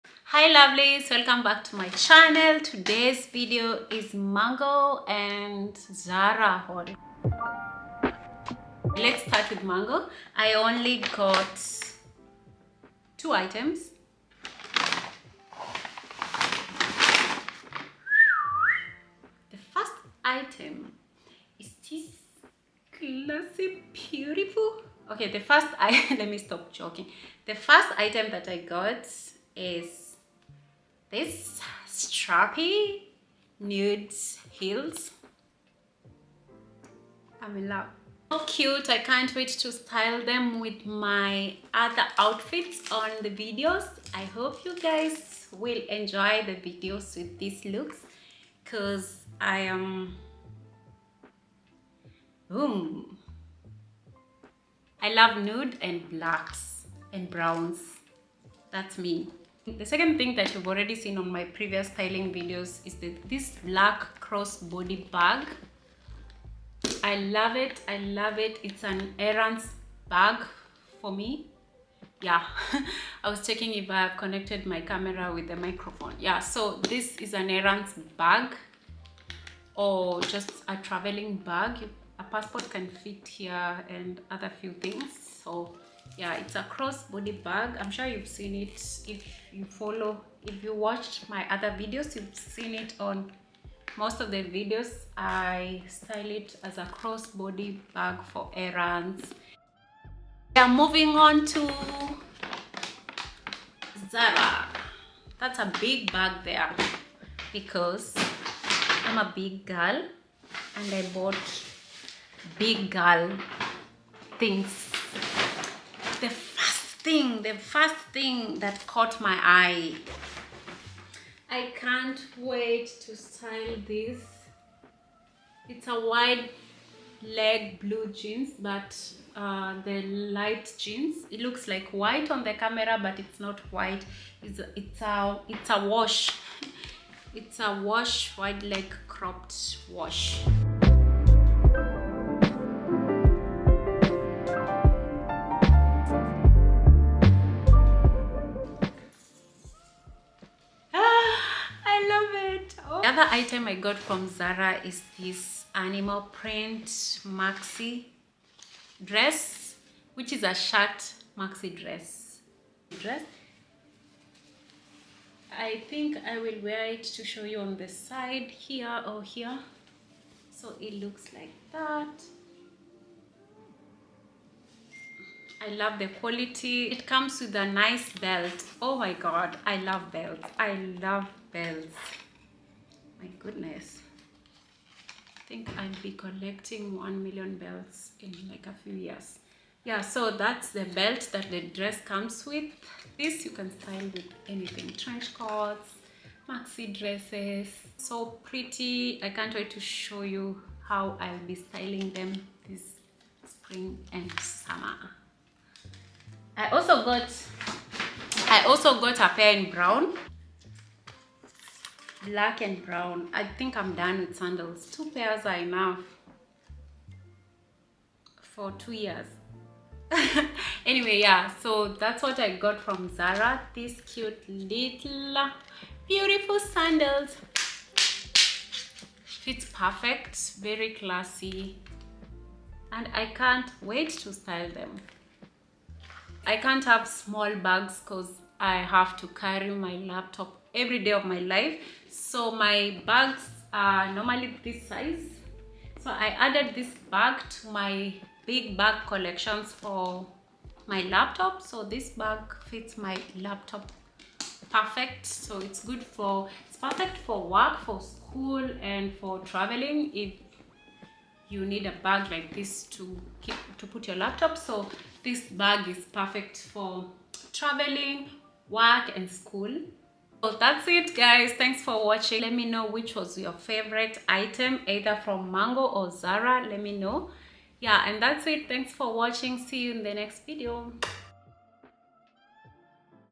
ZARA and MANGO try-on Haul - spring and summer essentials
I'm sharing my Zara and Mango spring haul with you! I picked up some amazing pieces that are perfect for the season. I hope you enjoy seeing what I got and get some inspiration for your own spring wardrobe!

BAG SIMILAR TO WHATS ON MY VIDEO.

ZARA ITEMS
1. WIDE LEG BLUE JEANS
5.  BAG: I couldn’t find it on ZARA website